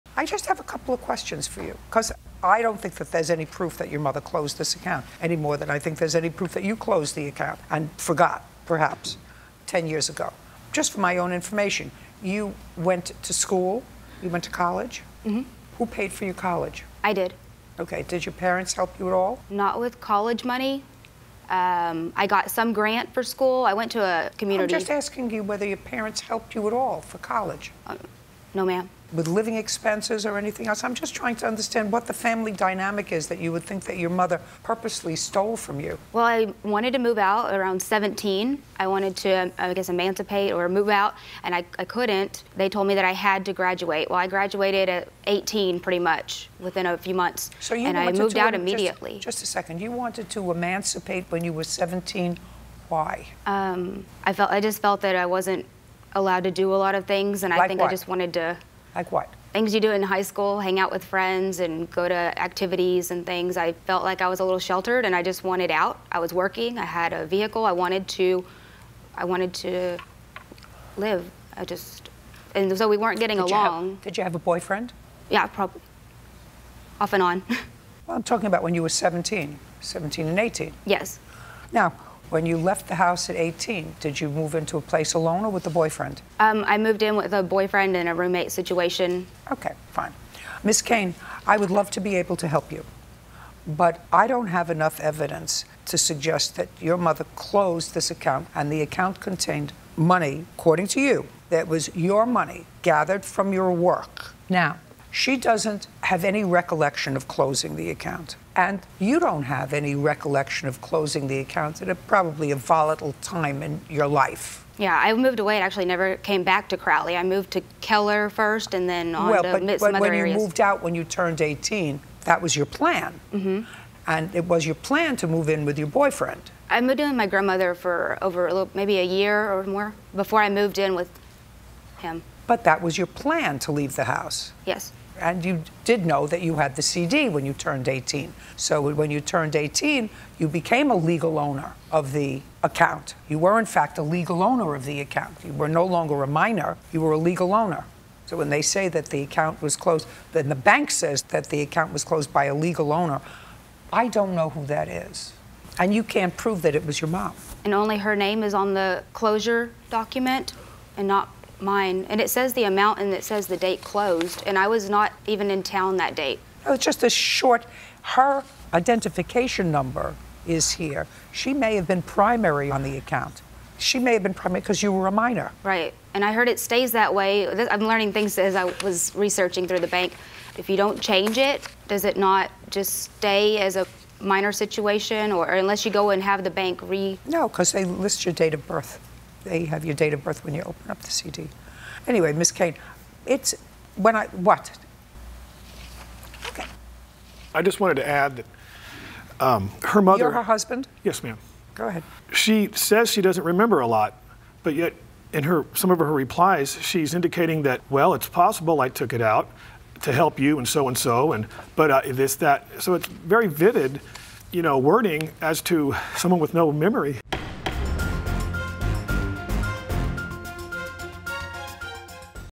Did Mom Steal Daughter's Childhood Savings? | Part 2
Ashley thinks her mother cashed out her childhood CD, but can she prove it?